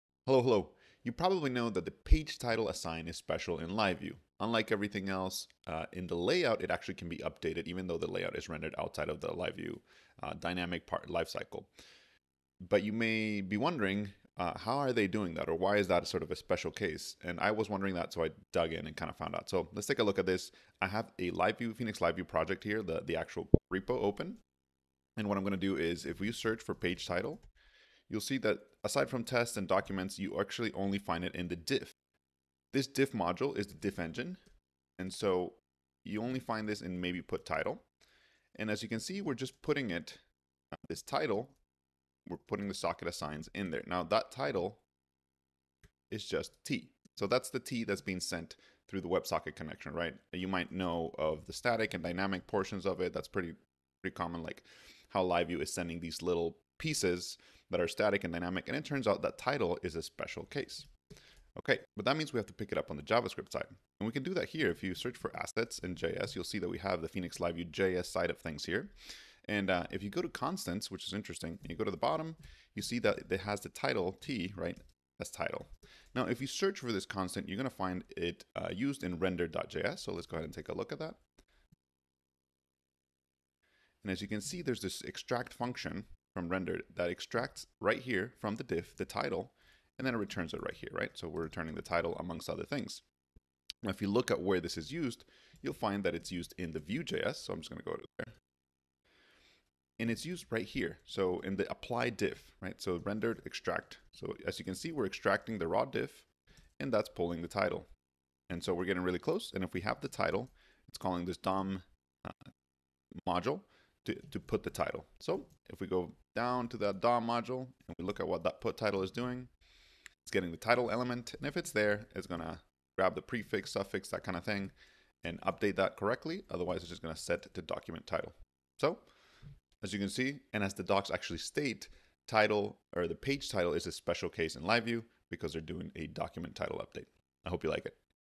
How is the `:page_title` assign updated?
You probably know that the `:page_title` assign is special in Phoenix -- it is the only dynamic assign in a layout. And I wondered, how does it work? How do they make it special? So, I did a bit of code spelunking.